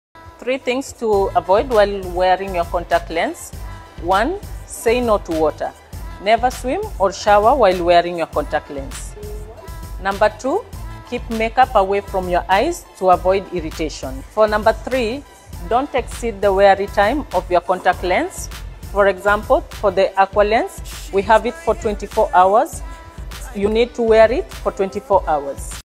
3 Things To AVOID While Wearing Contact Lenses
Do contact lenses also require care like glasses? Yes, absolutely they do.
They demand even more TLC to ensure impeccable hygiene and comfort. Watch on for a comprehensive rundown of all the things that you should avoid while wearing content lenses that will safeguard your precious peepers.